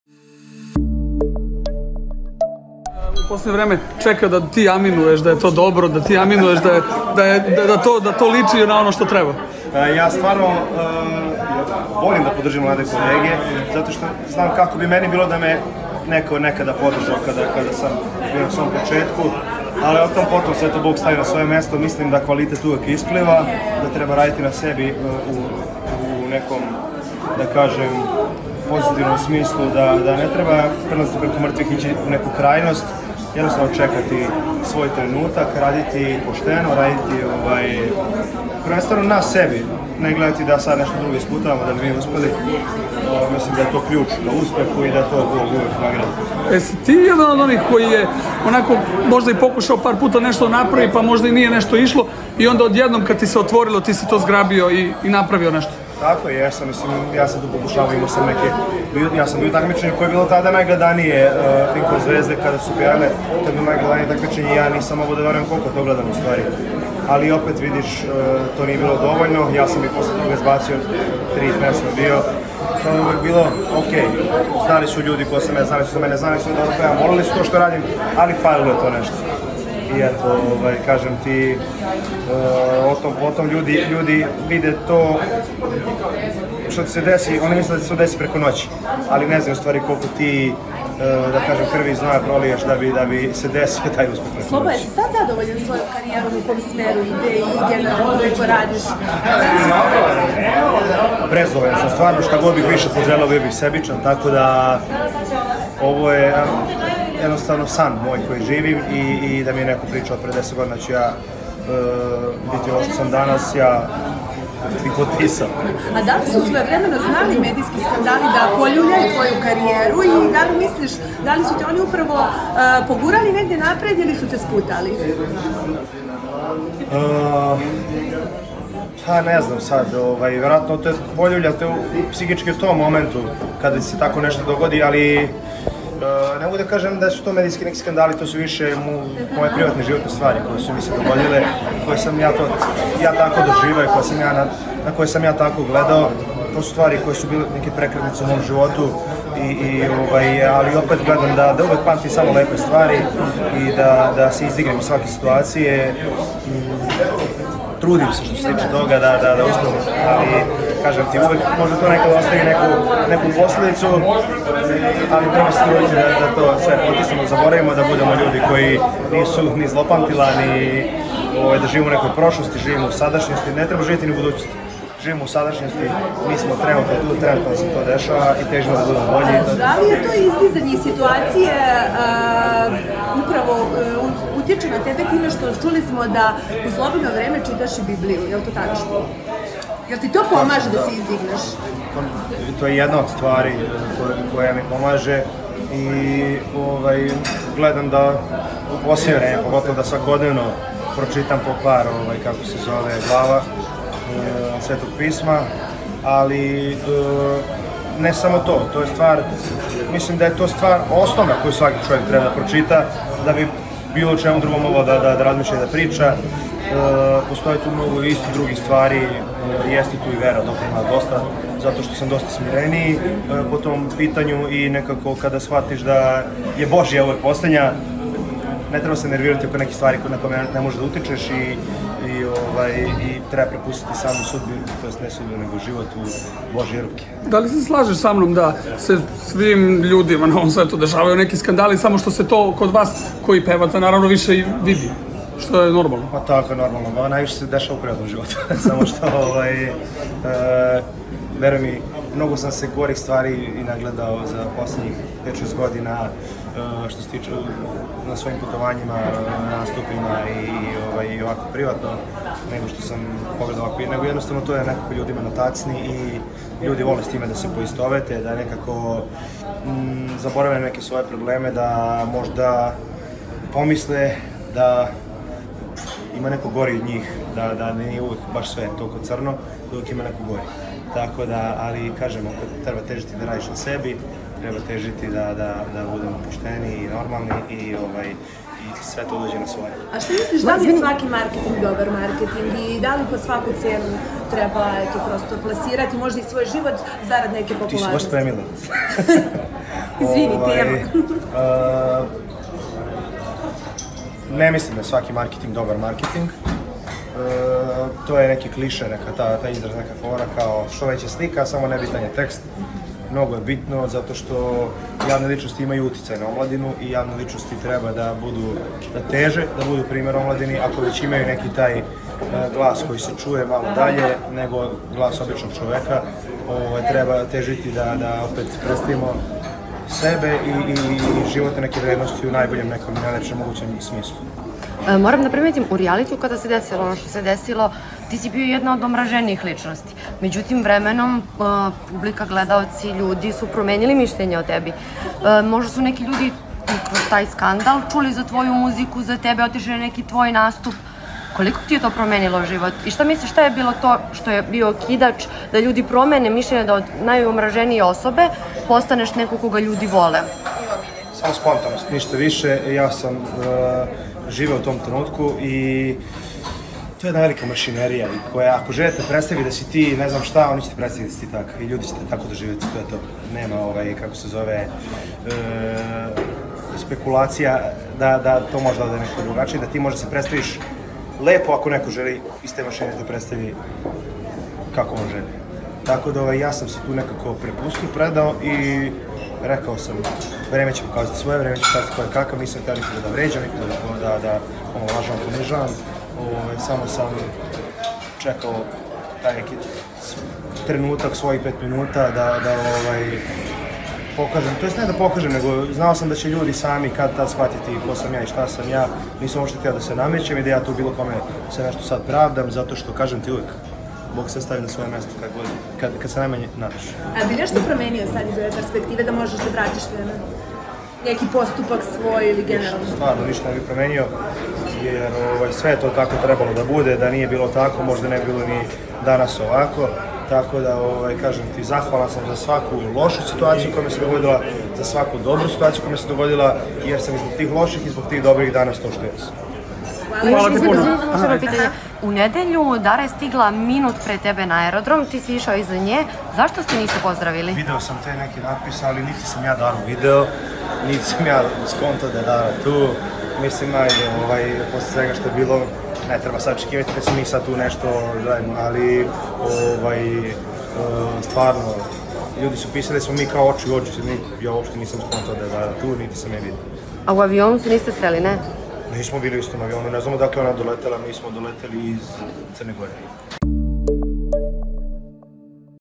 SLOBA OTVORENO O SUSTRETU SA DAROM NA AERODROMU Osvrnuo se i na varanje u rijalitiju
Folker Sloba Radanović otvorio je dušu o karijeri, čitanju Biblije, svom učešću u "Zadruzi" i koleginici Dari Bubamari.